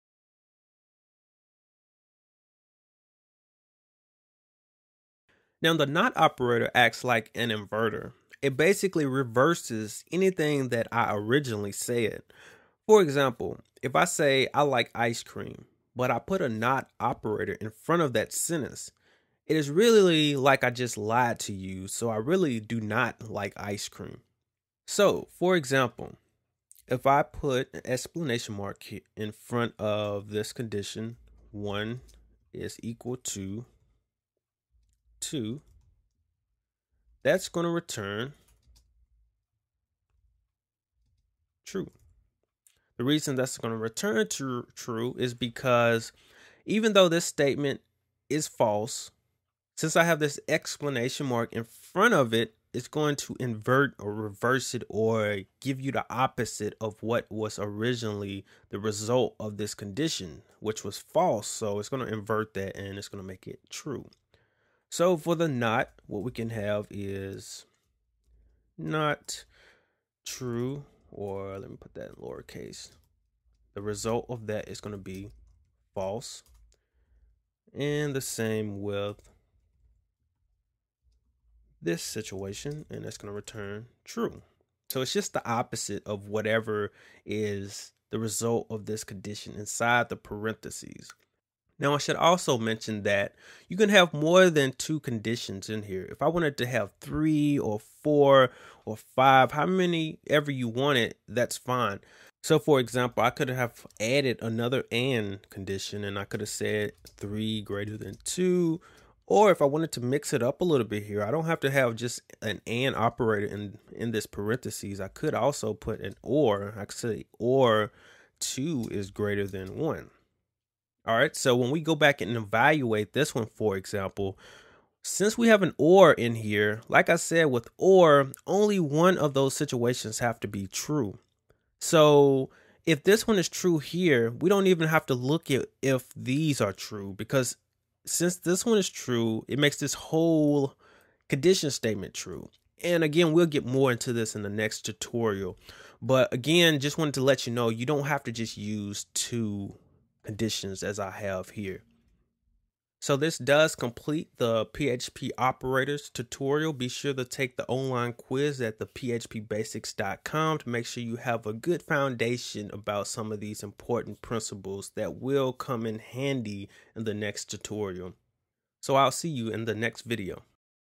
Tutorial 10 - PHP Operators - Part 2/2
Robert explains how to use PHP Operators.